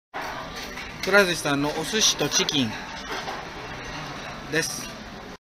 お食事処：くら寿司 大分中津店㊸ ～小林消防設備 福岡県豊前市 全類消防設備士 第二種電気工事士 経営学修士～
動画は、大分県中津市にあるくら寿司 大分中津店さんのお寿司とチキンです。
他にもいろいろな種類のメニューがあります。
ぜひ1度足をお運び下さい。
おすすめのお店です。